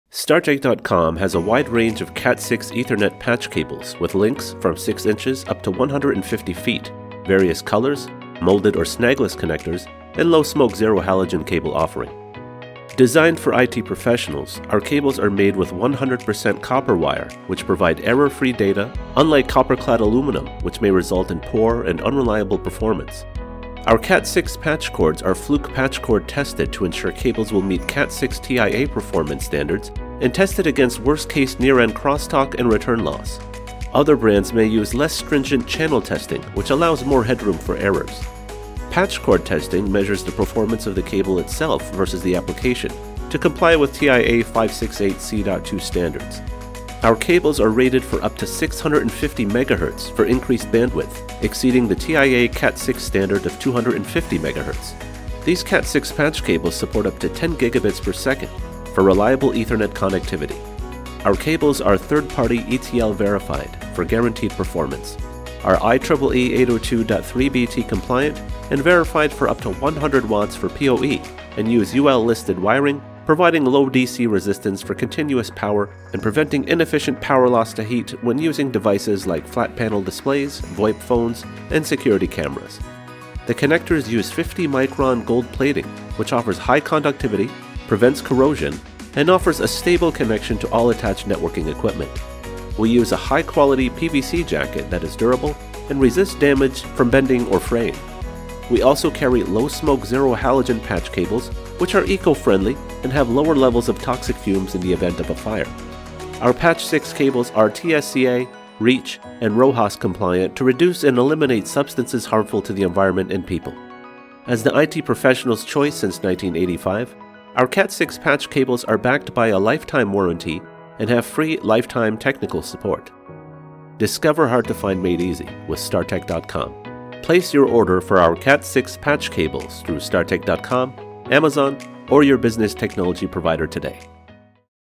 CAT6 Ethernet Patch Cables | StarTech.com
CAT6 Ethernet patch cables support up to 10Gbps and are Fluke Patch Cord Tested to provide reliable Ethernet connectivity.
We offer a comprehensive range of CAT6 network cables, available in lengths from 6”/15cm to 150ft/45m and in a variety of colors. Our CAT6 LAN cables are rated up to 100 watts for POE, making them ideal for devices such as flat panel displays, VOIP phones and security cameras. Options include molded or snagless connectors and Low Smoke Zero Halogen cables and are available in single and multipacks.
All of our CAT6 cables are TSCA, REACH and RoHS compliant and come with a lifetime warranty and technical support.
Key Features:
1.     Supported: 1Gbps, 2.5Gbps, 5Gbps, 10Gbps (up to 55m), 10GBase-T
2.     Support POE applications up to 100W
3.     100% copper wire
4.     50-micron gold plated connectors
5.     Fluke Patch Cord Tested
6.     Third party ETL verification
7.     Compliance with IEEE 802.3bt and TIA-568-C.2 standards
8.     Low Smoke Zero Halogen and molded or snagless connector models

THE IT PRO'S CHOICE: Designed and built for IT Professionals, our CAT6 Ethernet patch cables are backed by a lifetime warranty, including free lifetime 24/5 multi-lingual technical assistance.